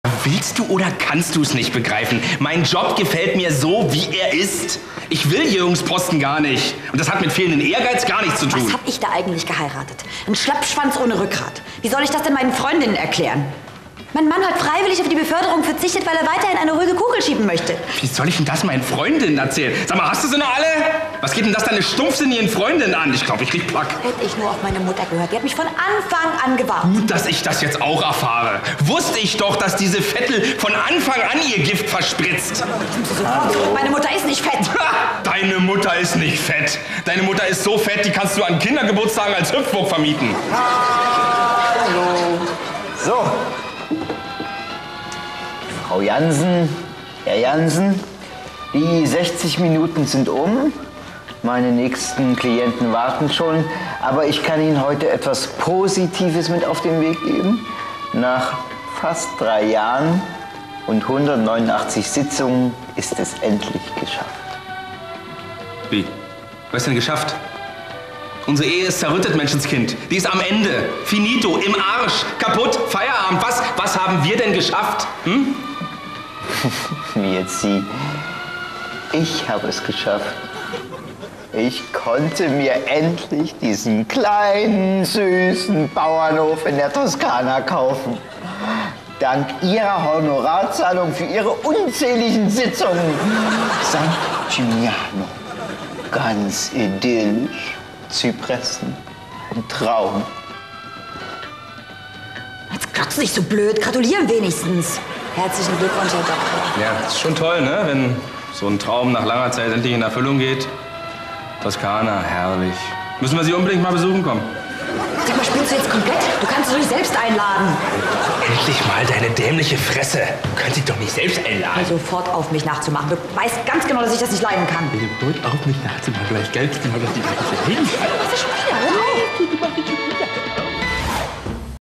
Paartherapie
streitendes Ehepaar beim Paartherapeuten